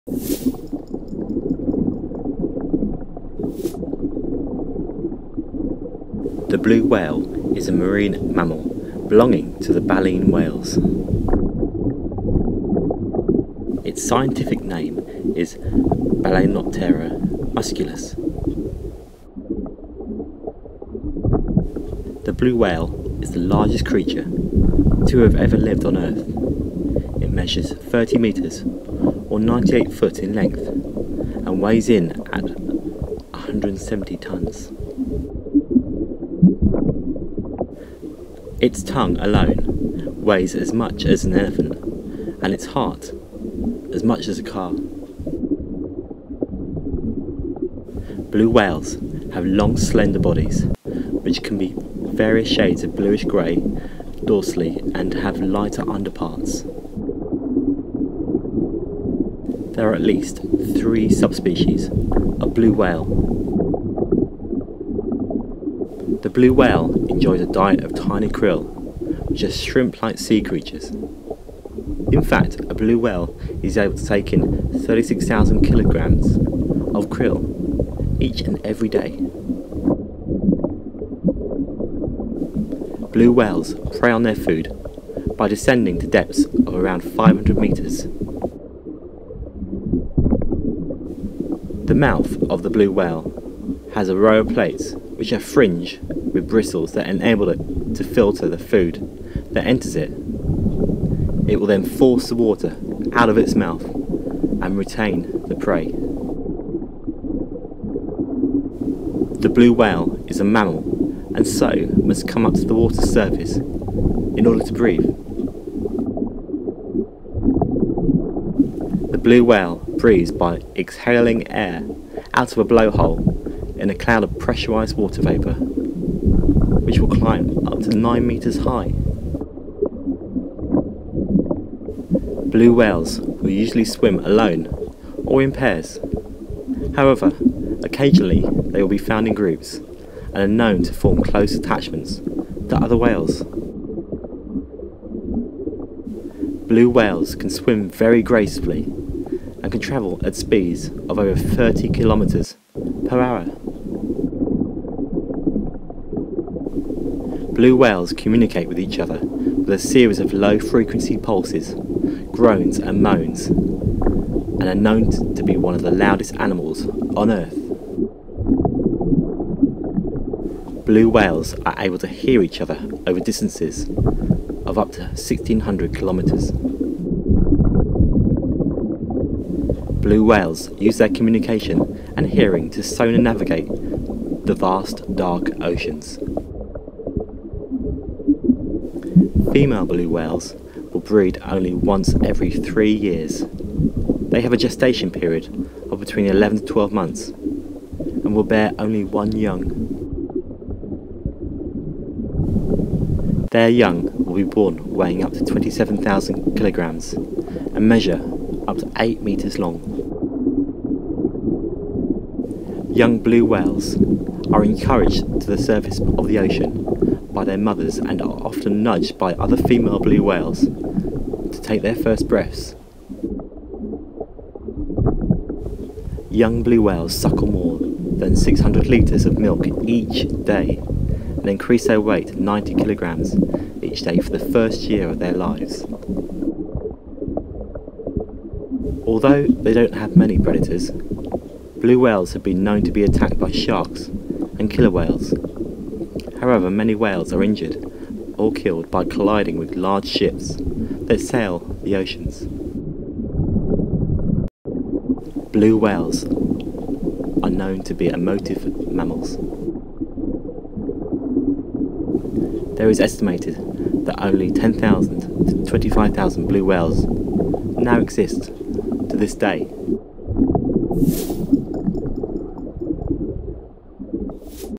Blue Whale Facts: 23 facts about the Blue Whale
Here are 23 facts about the blue whale to help gain a better understanding of this wonderful mammal known as the largest creature to have ever lived.

Synopsis

23. The blue whale is a marine mammal belonging to the baleen whales.

22. It's scientific name is Balaenoptera musculus

21. The blue whale is the largest creature to have ever lived on earth measuring 30 metres or 98 ft in length and weighing in at 170 tonnes

20. The tongue alone weighs as much as an elephant and it's heart as much as a car.

19. Blue whales have long slender bodies which can be various shades of bluish-grey dorsally and have lighter underparts.

18. There are at least three subspecies of blue whale.

17. The blue whale enjoys a diet of tiny krill which are shrimp like sea creatures. In fact a blue whale is able to take in 3,6000kg of krill per day.

16. Blue whales prey on their food by descending to depths of around 500m.

15, The mouth of the blue whale has a row of plates which are fringed with bristles that enable it to filter the food that enters it. It will then force the water out of its mouth and retain the prey.

14. The blue whale is a mammal and so must come up to the waters surface in order to breathe.

13. The blue whale breathes by exhaling air out of a blowhole in a cloud of pressurised water vapour which will climb up to 9 meters high.

12. Blue whales will usually swim alone or in pairs, however occasionally they will be found in groups and are known to form close attachments to other whales.

11. Blue whales can swim very gracefully and can travel at speeds of over 30km/h.

10. Blue whales communicate with each other with series of low frequency pulses, groans, and moans and are known one of the loudest animals on earth.

9. Blue whales are able to hear each over distances of up to 1,600km.

8. Blue whales use their communication and hearing to sonar-navigate the vast dark oceans

7. Female blue whales will breed only once every three years. They have a gestation period of between 11-12 months and will bear only one young.

6. Their young will be born weighing up to 2,7000kg and measure up to 8m long.

5. Young blue whales whales are encouraged to the surface of the ocean by their mothers and are often are nudged by other female blue whales to take their first breath.

4. Young blue whales suckle more than 600 litres of milk each day and increase their weight 90kg each day for the first year of their lives.

3. Although they don't have many predators, blue whales have been known to be attacked by sharks and killer whales. However many whales are injured or killed by colliding with large ships that sail the oceans.

2. Blue whales have are known to be emotive mammals.

1. There is estimated that only 10,000-25,000 blue whales now exist to this day.